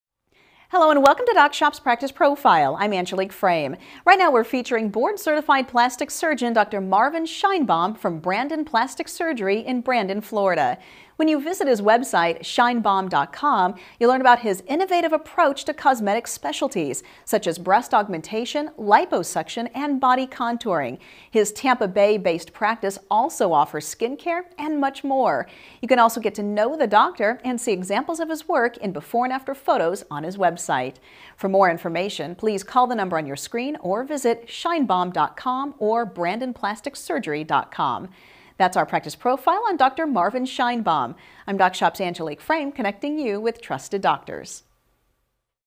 Dr. Marvin F. Shienbaum - Plastic Surgery in Brandon near Tampa Bay, Florida
Dr. Marvin F. Shienbaum from Brandon Plastic Surgery is an experienced board certified plastic surgeon in Brandon, Florida. Dr. Shienbaum specializes in breast augmentation, liposuction, and body contouring. His Tampa Bay-based practice also offers skin care, and much more. For more information about the procedures, or to contact Dr. Shienbaum, please

You may contact Dr. Marvin F. Shienbaum at:

Brandon Plastic Surgery
500 Vonderburg Dr. West
Suite 110